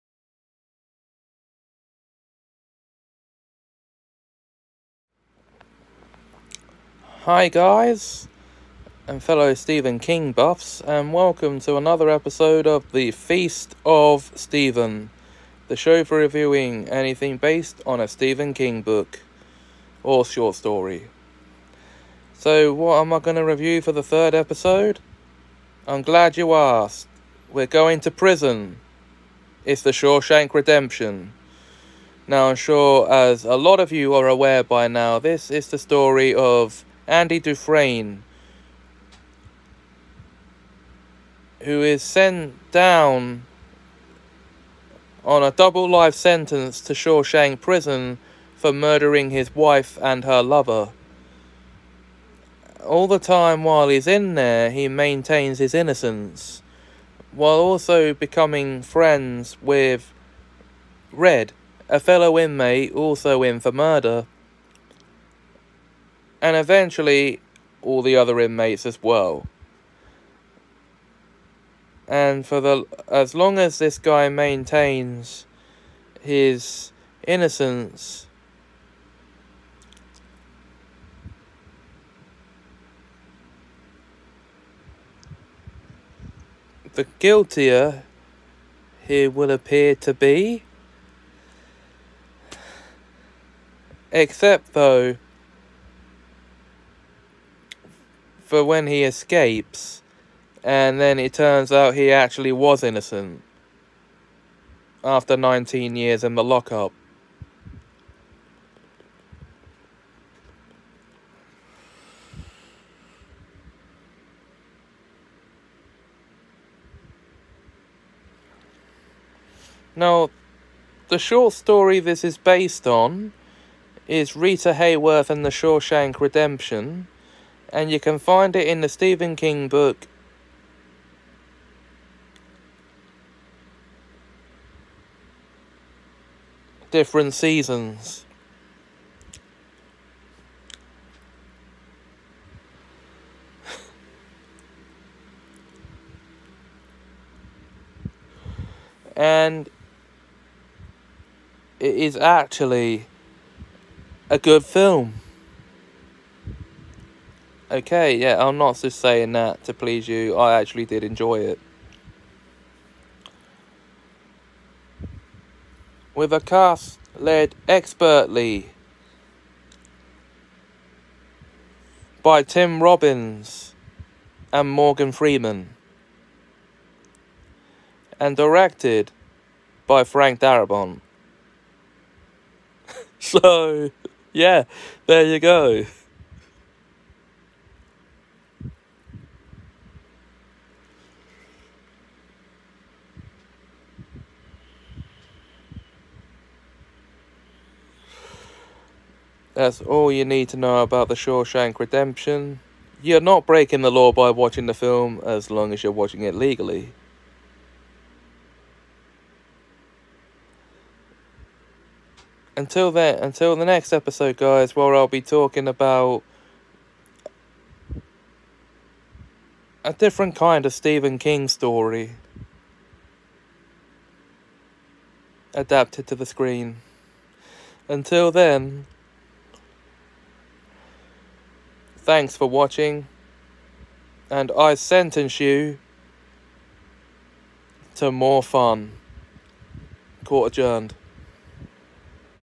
The Feast of Stephen, Episode 3: The Shawshank Redemption
The Feast of Stephen: Series 1, Episode 3: I review the 1994 prison drama “The Shawshank Redemption”